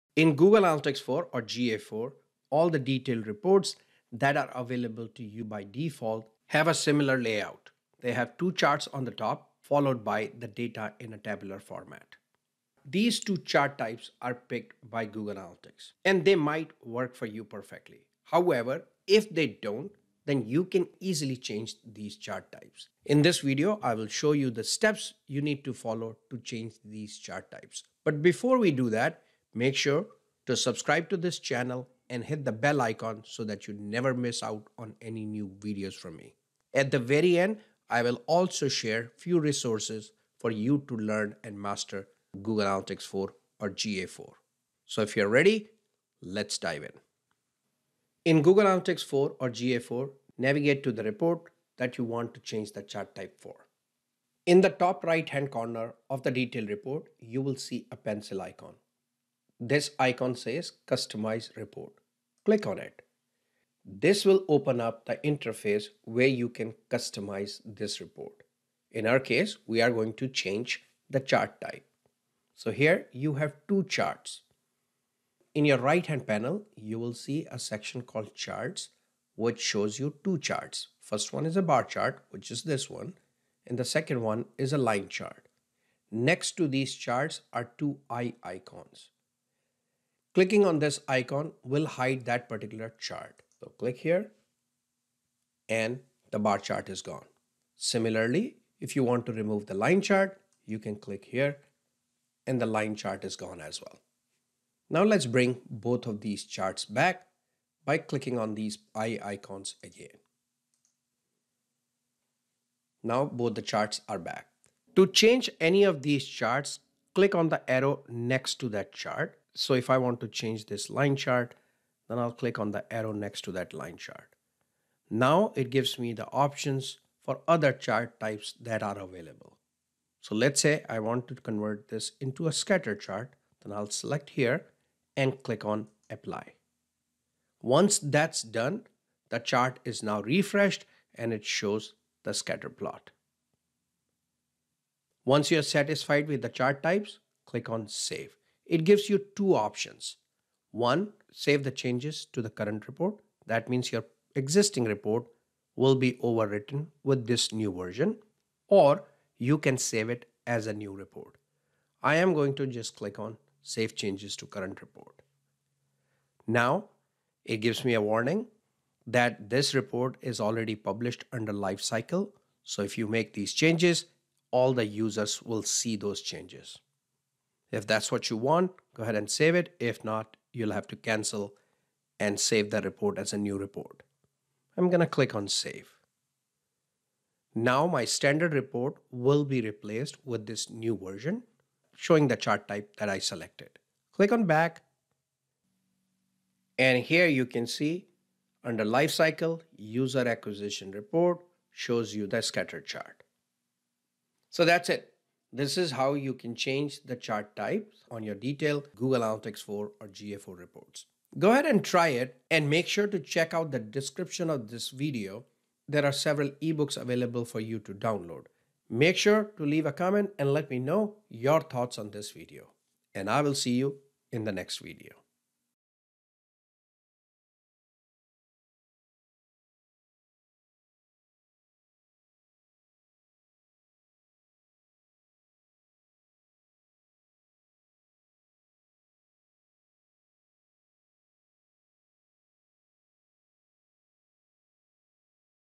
GA4 - How to change Chart Types in Google Analytics 4 Reports - GA4 report customization.
Learn how to change Chart types Google Analytics 4 reports - GA4 report customization.

This video is part of GA4 tips and tricks series